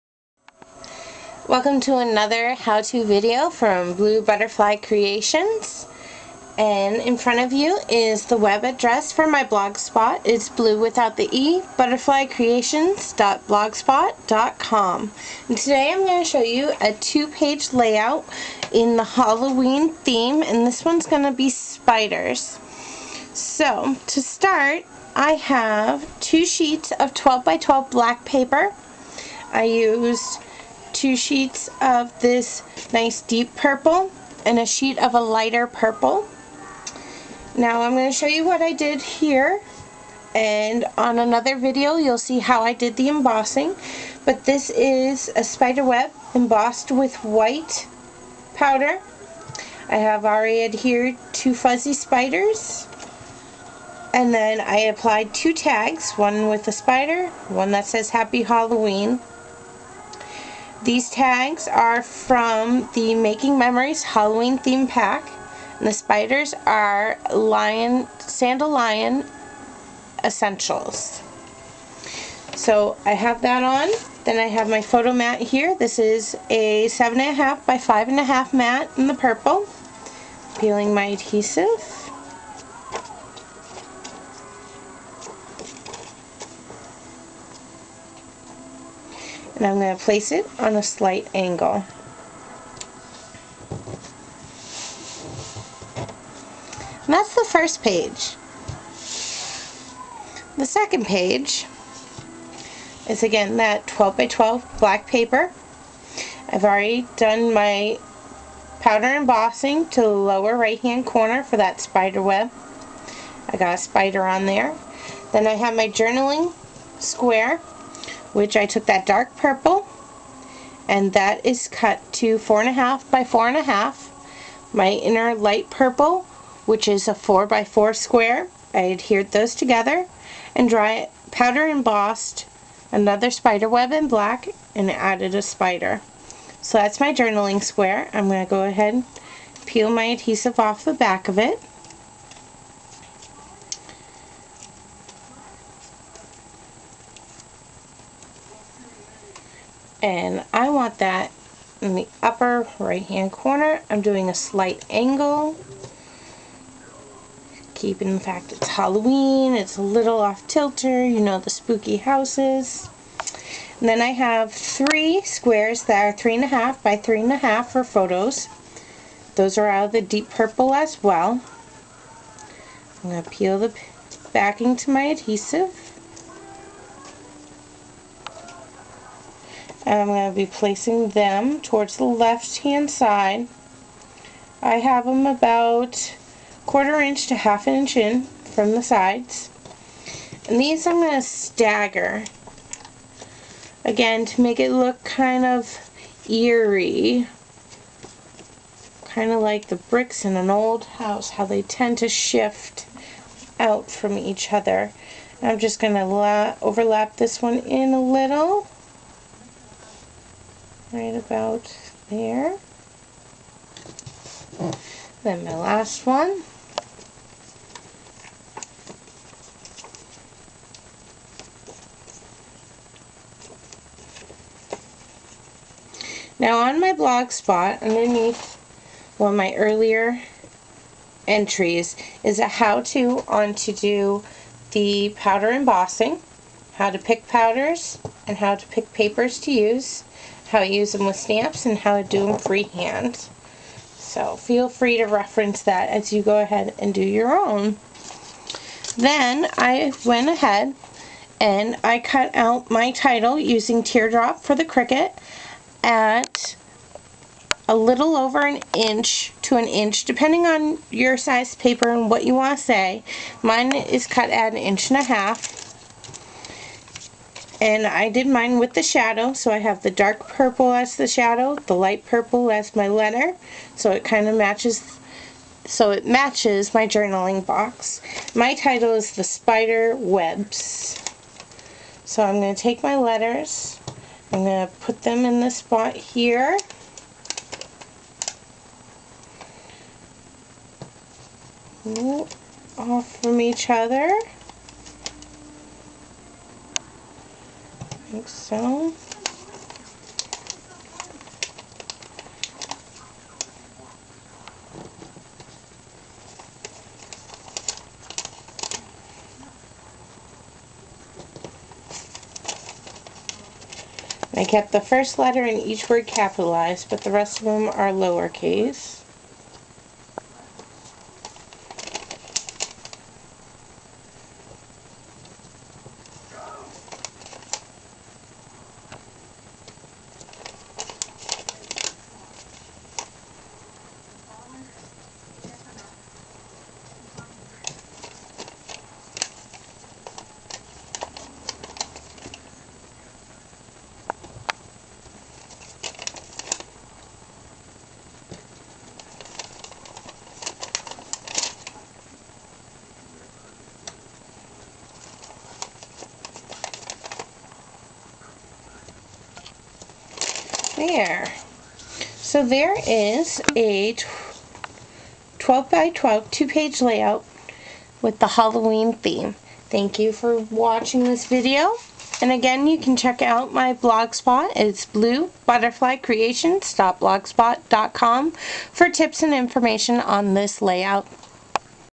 Embossed 2page Spider Theme Lay-out
A how-to video for creating a 2 page lay-out using powder embossed spider webs. Please refer to the Embossing with Powder Spider Webs video for initial steps.

This video and it's content is not made for children.